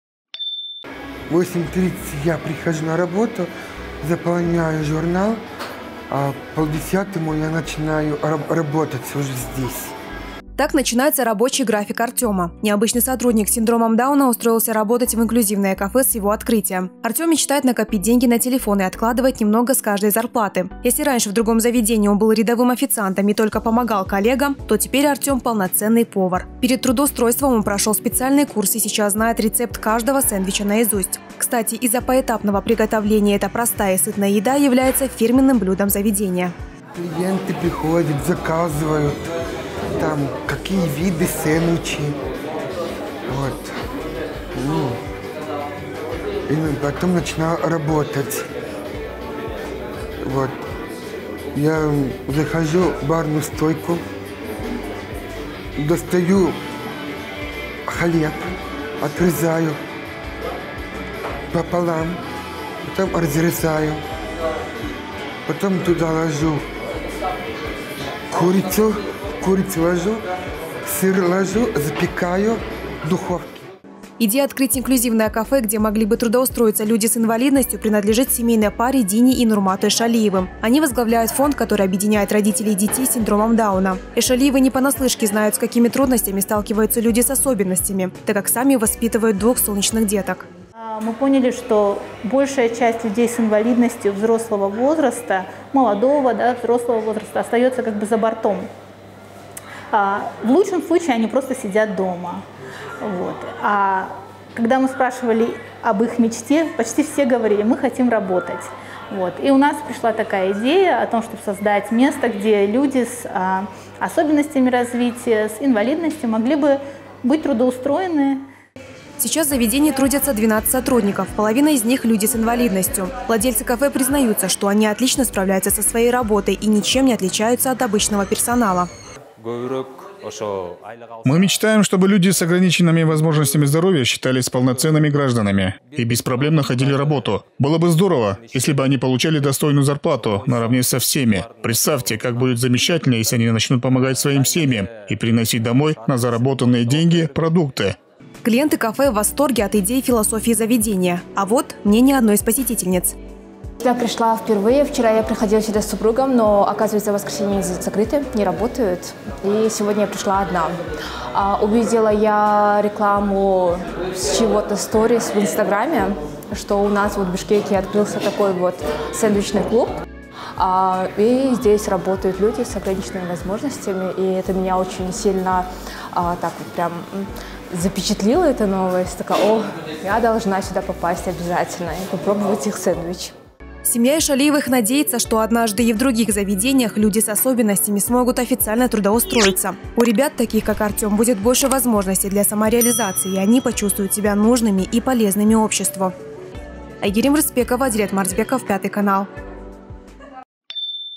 Инклюзивное кафе
В столице открылось необычное кафе. У заведения есть особая уникальность. Там работают сотрудники с ограниченными возможностями здоровья. Какое фирменное блюдо в меню инклюзивного кафе и о дальнейших планах владельцев заведения - узнавала Айгерим Рысбекова.